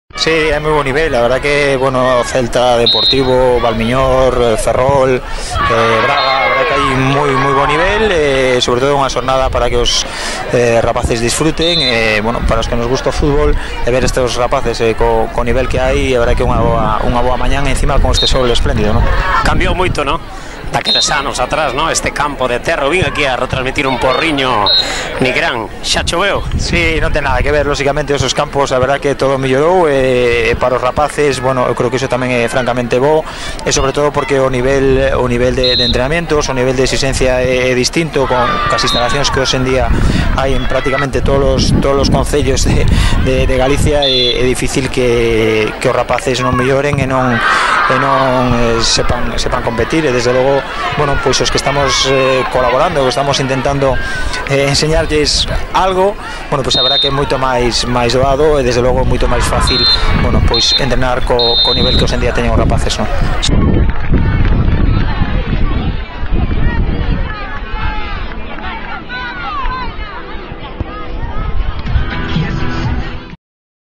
VAL MIÑOR CUP EN MINUTO 91,NEL FERNANDEZ CON XURXO OTERO,BETIS,AT MADRID,RC CELTA...
PENALTIS,DEPOR VENCE A VAL MIÑOR.